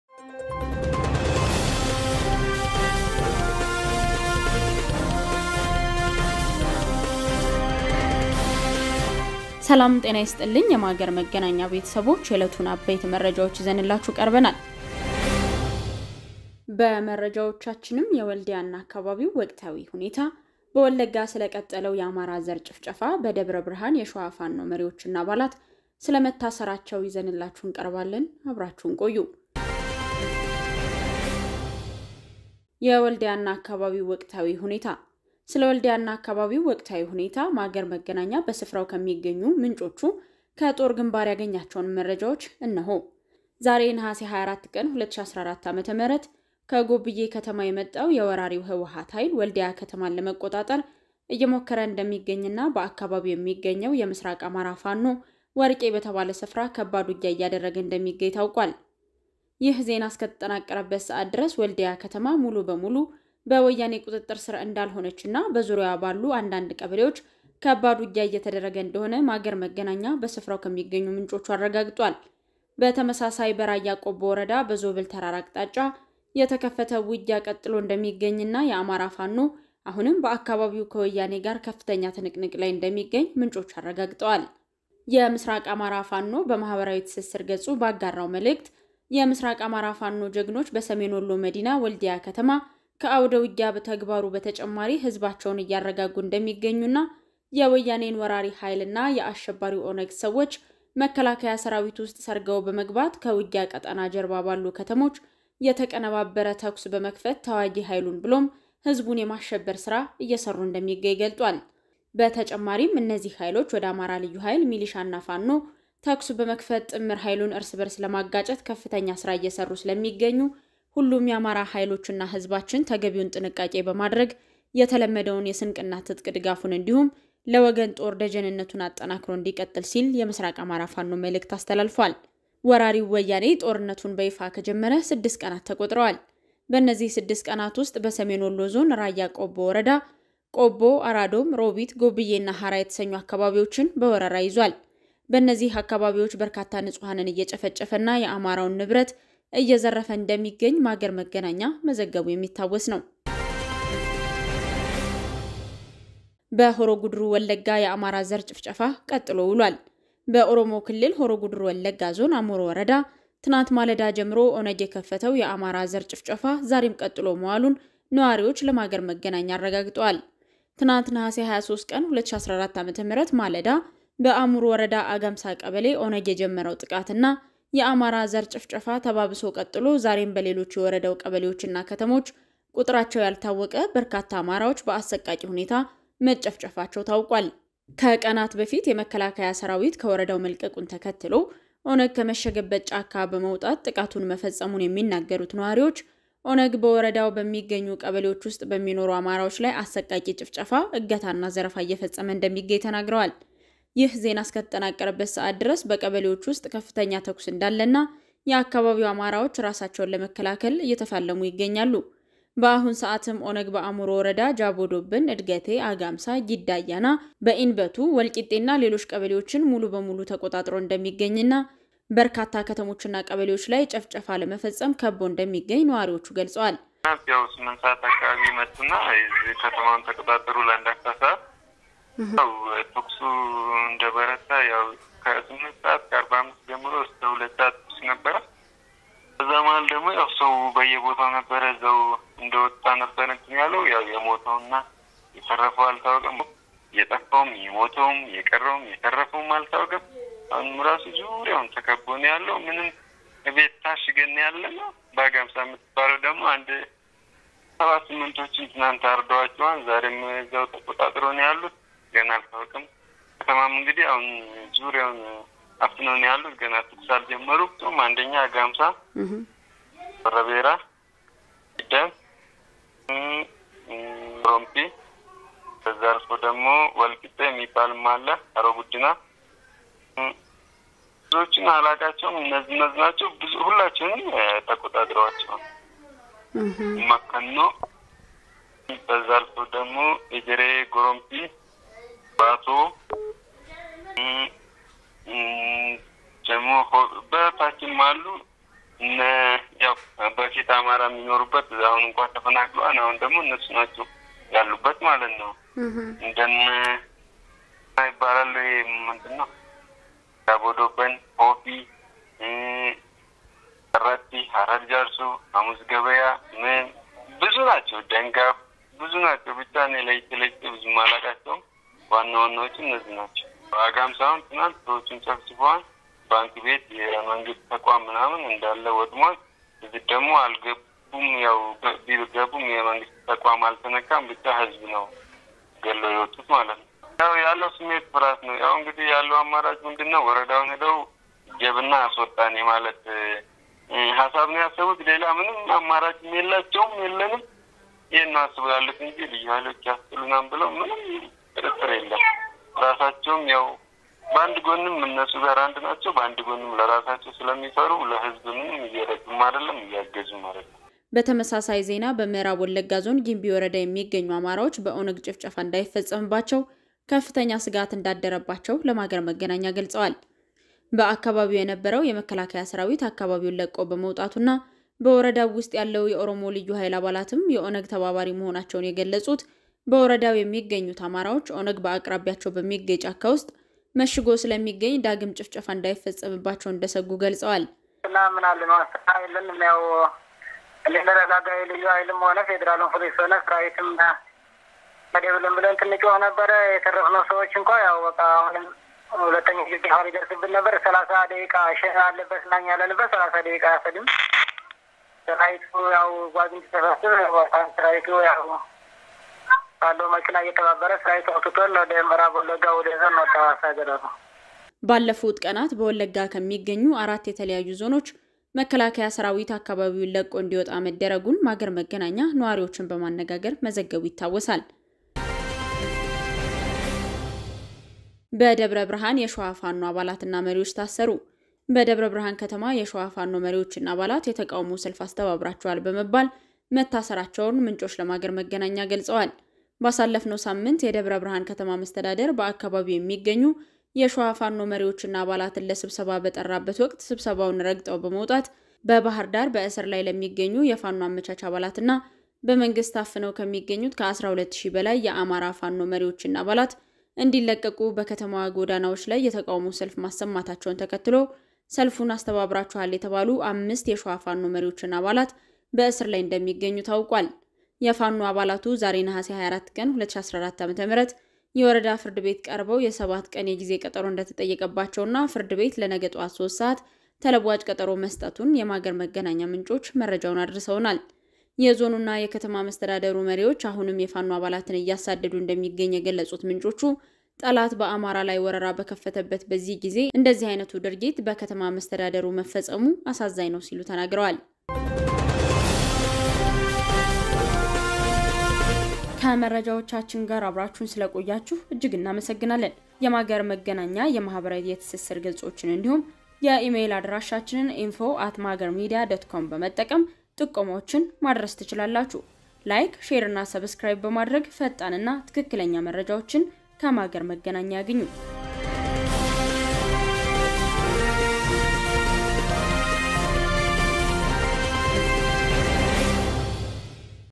ማገር እለታዊ ዜና 08-30-2022
• የወልድያና አካባቢው ወቅታዊ ሁኔታ
• በወለጋ የቀጠለው የአማራ ዘር ጭፍጨፋ
• በደብረ ብርሃን የፋኖዎች እስር